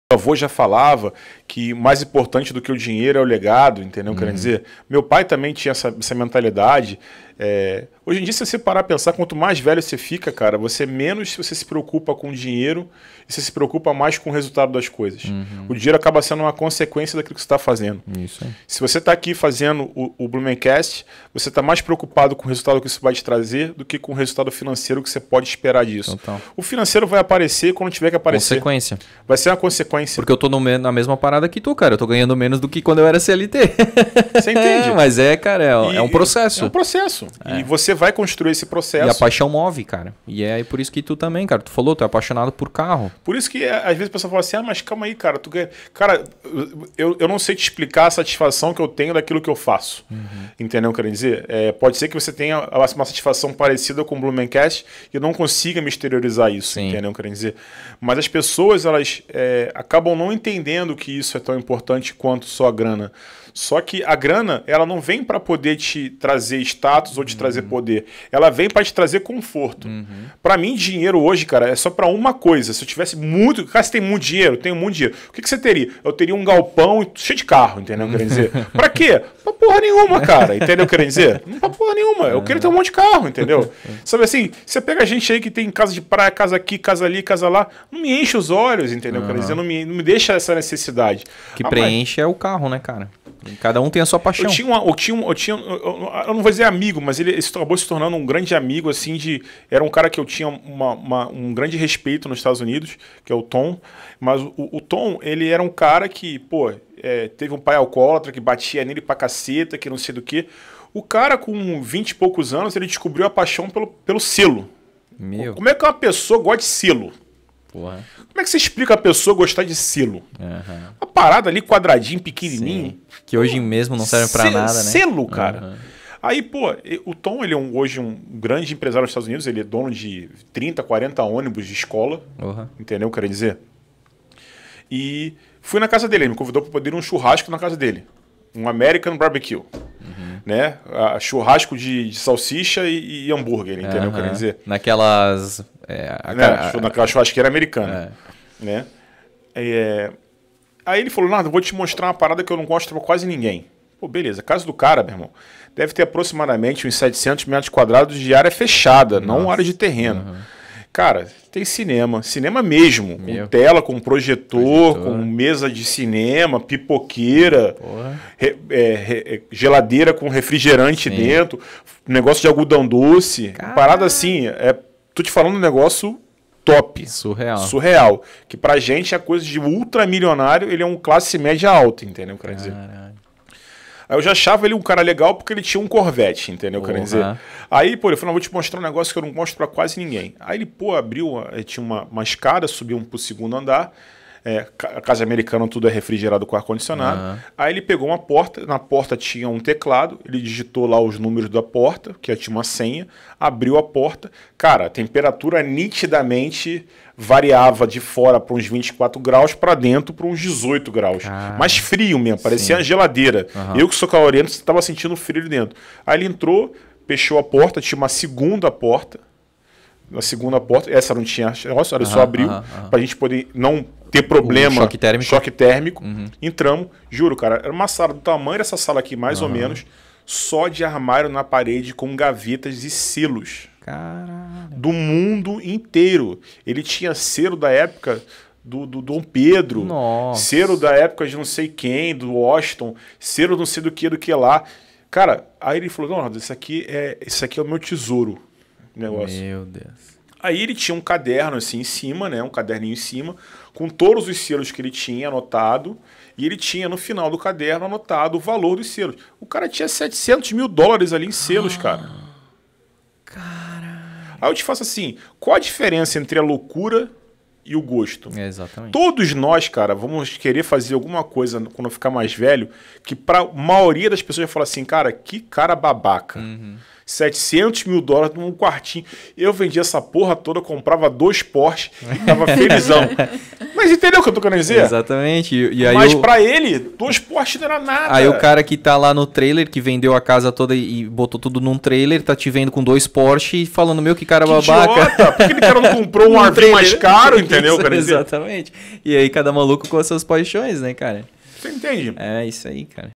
A PAIXÃO POR UM HOBBY PODE SER EXPLICADA?
Convidado: Leonardo (Naza Auto Service)

Host: André Cantoni

Siga a gente no instagram: @blumencast